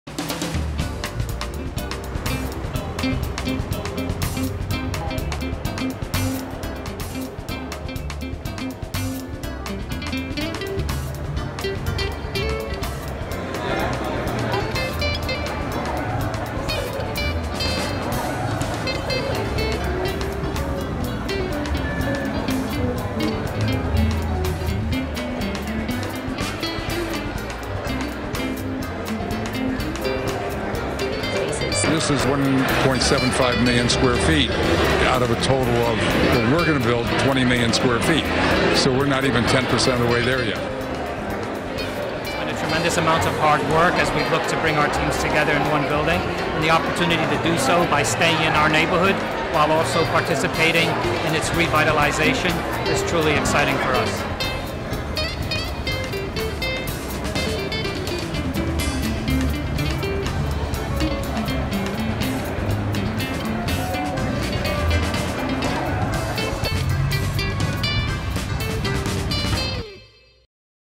The Opening of 10 Hudson Yards
On May 31st, 2016, Hudson Yards celebrated the opening of its first building, 10 Hudson Yards, which its first tenants, Coach Inc.